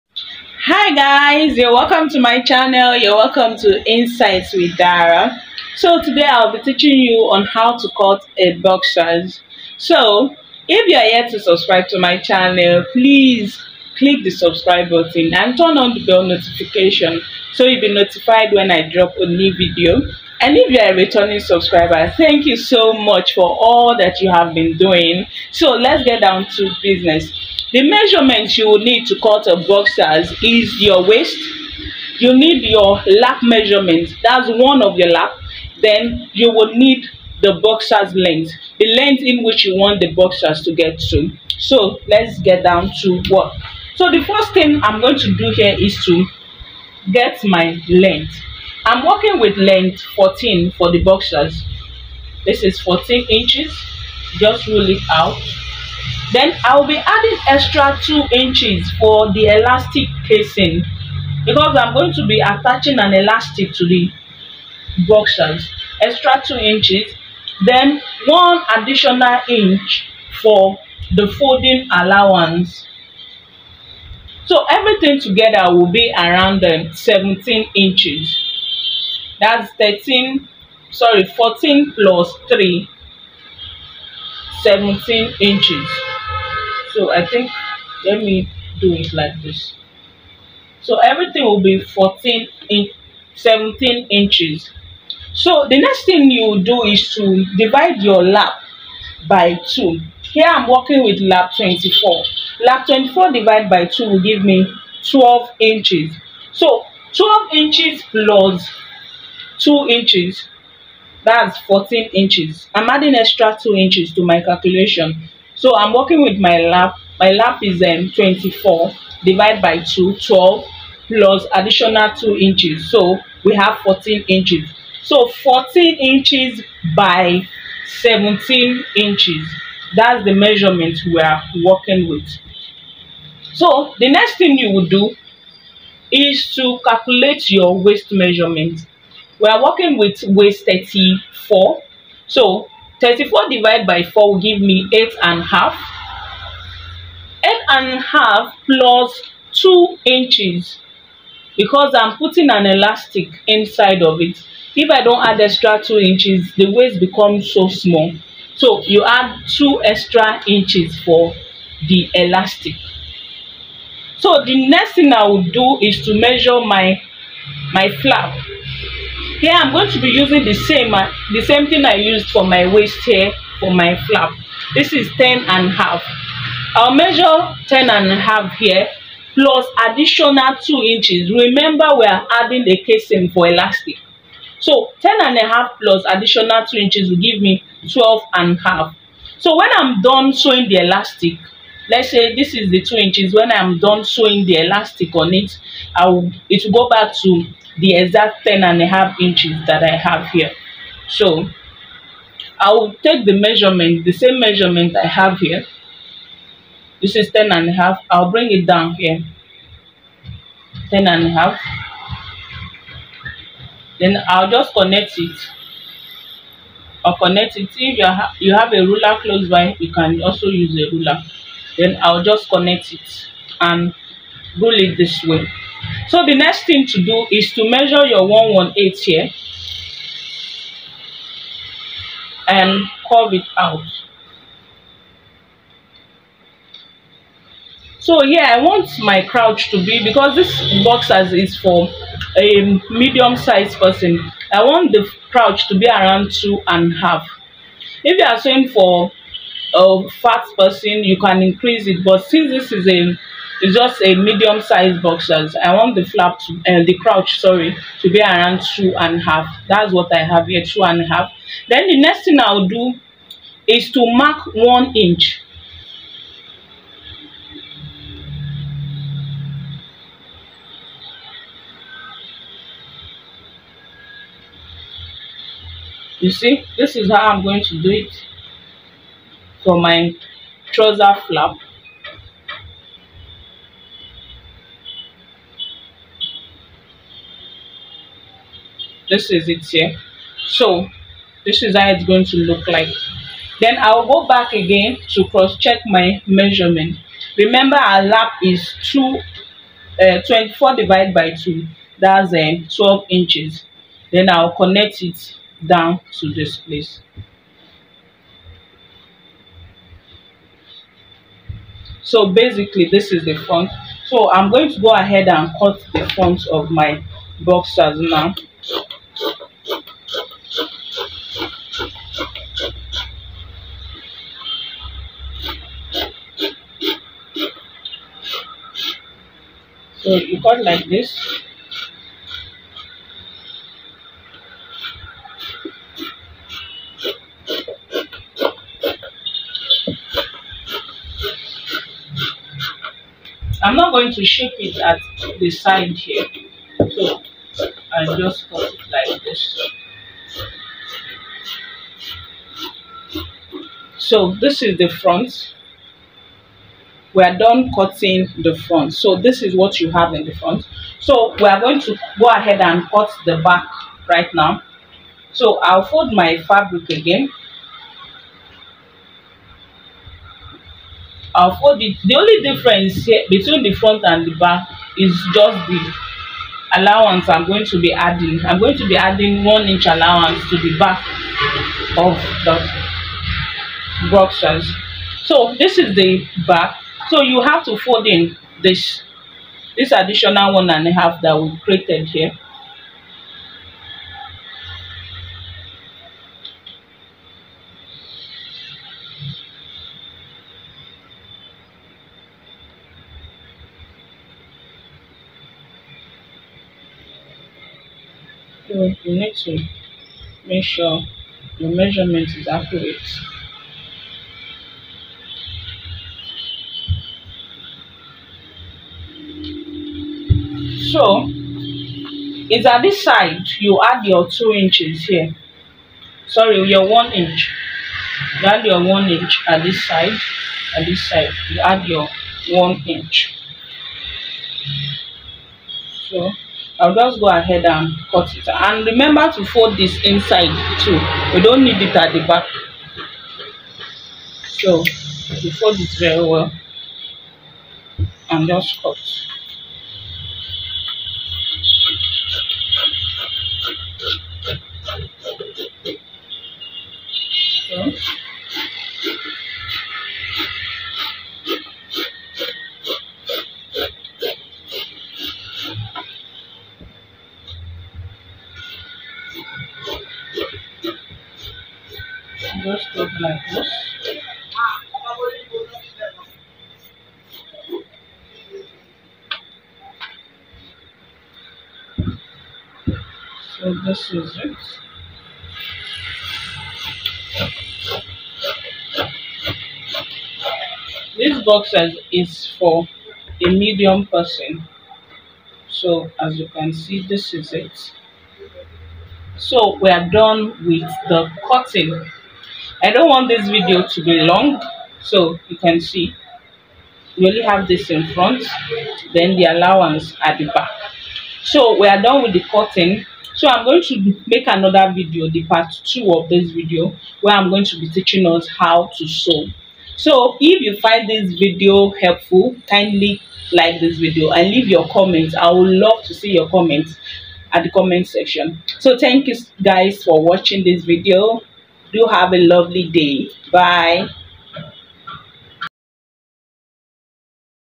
How To Cut and Sew A Boxer For Beginners.Detailed Tutorial For Boxers Making.
Are you a beginner eager to master the art of making your very own boxers from scratch? 🩳✂️ Look no further! In this comprehensive tutorial, we'll take you step-by-step through the process of cutting and sewing your own comfortable and stylish boxers.

From selecting the right fabric to sewing techniques and pattern tips, we've got you covered. Whether you're a sewing novice or looking to refine your skills, this tutorial will empower you to create custom boxers that fit you perfectly

Key Highlights:

 🧵 Choosing the Ideal Fabric 📏 Measuring and Cutting Patterns 🪡 Sewing Techniques for Beginners ✅ Detailed Step-by-Step Instructions 🎨 Customization and Design Tips

DIY Boxers

Join us on this creative journey and become a boxer-making pro in no time! Whether it's for personal use or as a thoughtful gift, your handmade boxers will be a hit. Don't forget to use sewingtutorial to share your creations and connect with fellow sewing enthusiasts.

want to know how to start a boxers ready to wear business